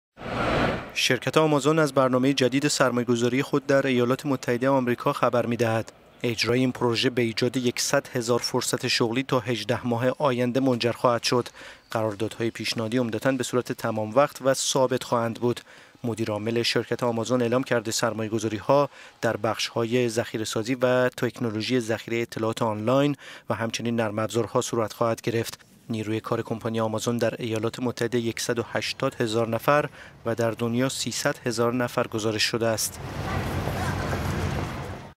اشتغالزایی بالای کمپانی آمازون در آمریکا - economy
شرکت آمازون از برنامه جدید سرمایه گذاری خود در ایالات متحده آمریکا خبر می دهد.

اجرای این پروژه به ایجاد یکصد هزار فرصت شغلی تا ۱۸ ماه آینده منجر خواهد شد. قراردادهای پیشنهادی عمدتا بصورت تمام وقت و ثابت خواهند بود.

مدیرعامل شرکت آمازون اعلام کرده سرمایه گذاریها در بخشهای ذخیره سازی و تکنولوژی ذخیره اطلاعات آنلاین و همچنین نرم افزارها صورت خواهد گرفت.

نیروی کار کمپانی آمازون در ایالات متحده ۱۸۰ هزار نفر و در دنیا ۳۰۰ هزار نفر گزارش شده است.…